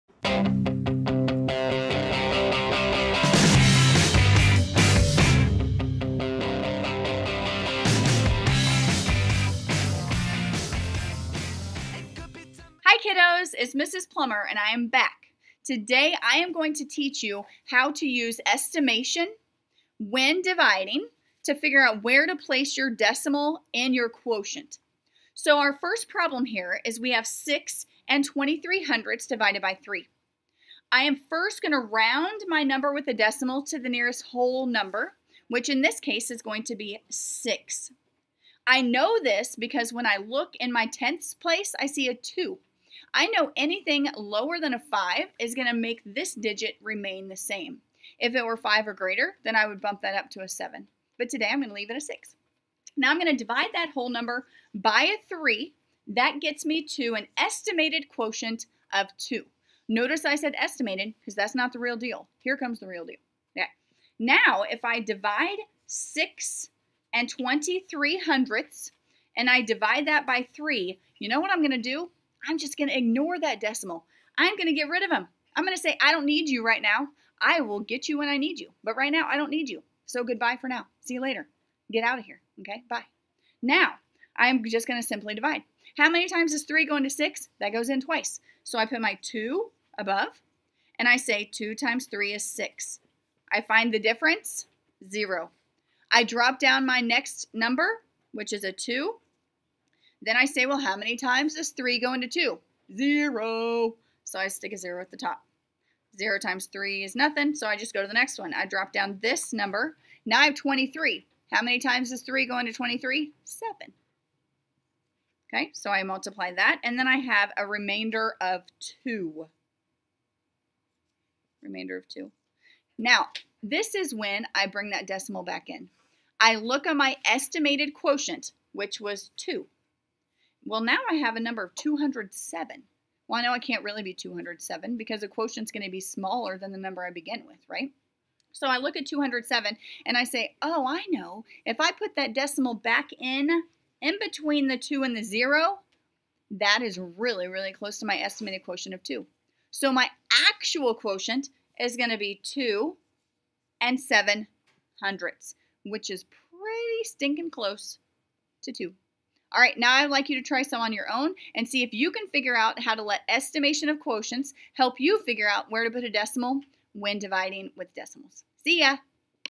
Estimation with Division of Decimals
How using an estimation can help verify our quotient when dividing decimals